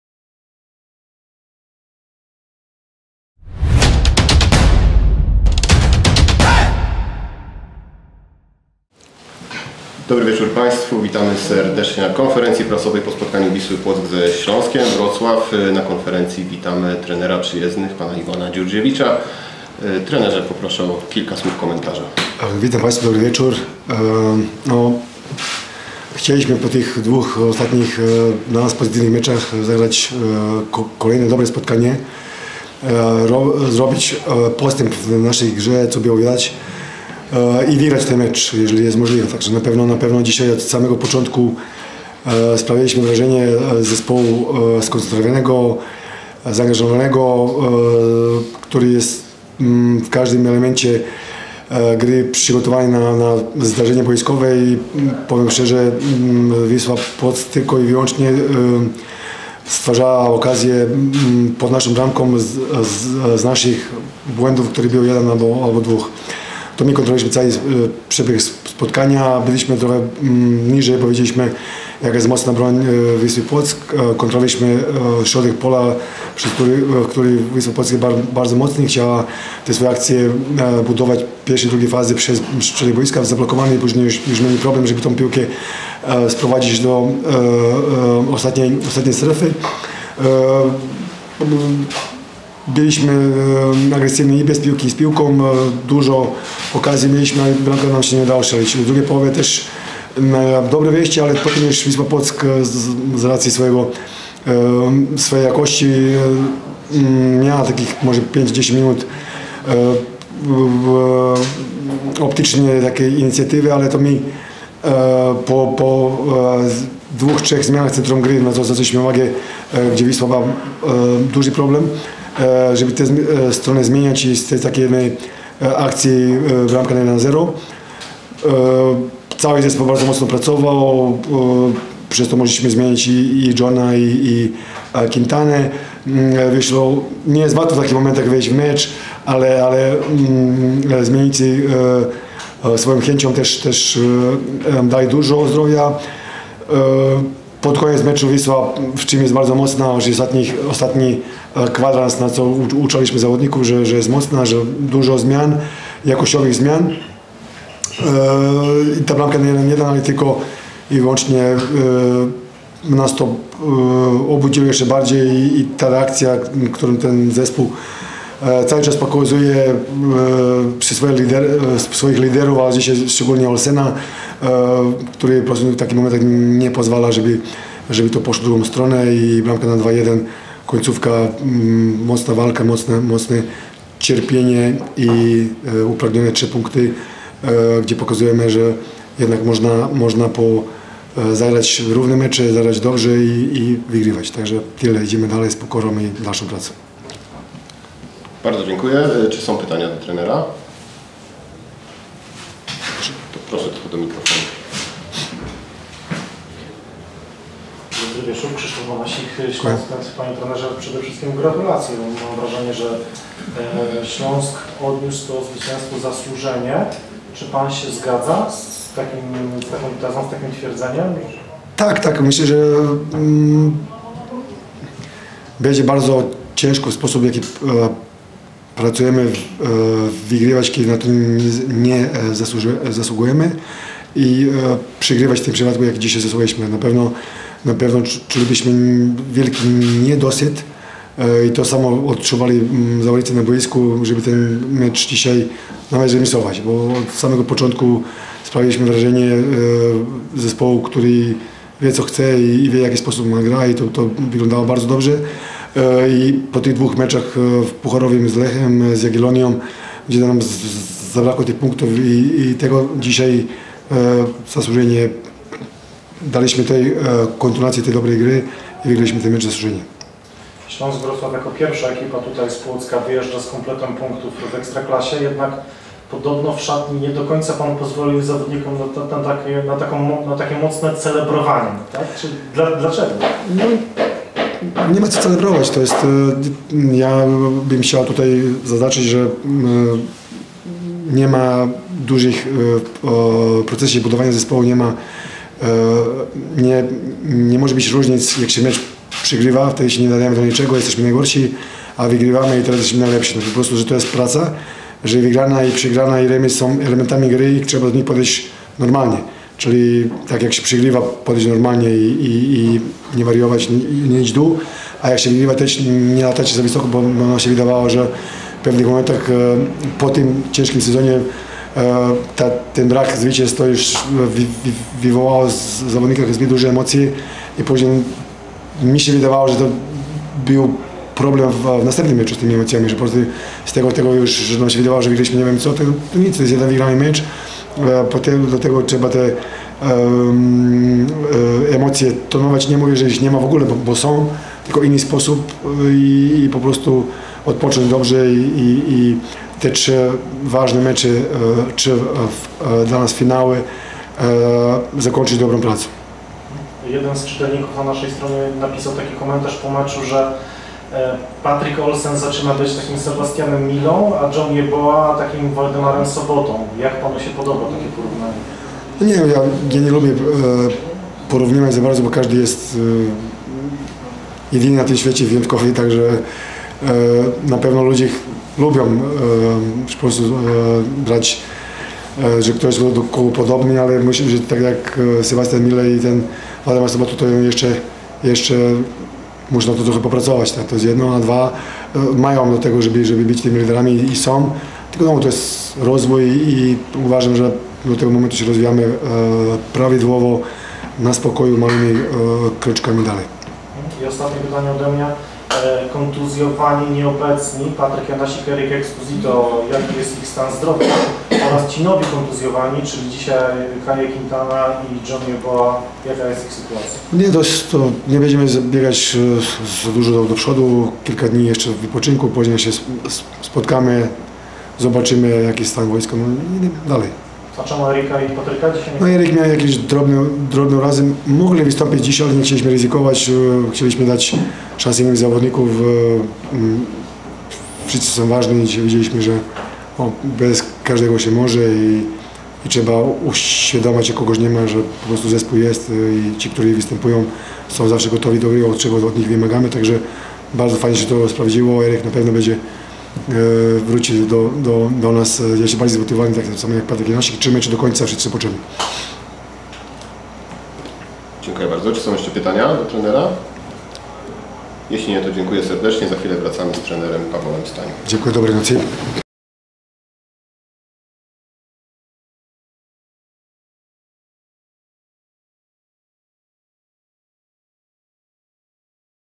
Ivan Djurdjević po meczu Wisła Płock - Śląsk Wrocław
Śląsk Wrocław pokonał Wisłę Płock (2:1) w meczu 15. kolejki PKO BP Ekstraklasy. Prezentujemy zapis pomeczowej konferencji prasowej z udziałem trenera Ivana Djurdjevicia.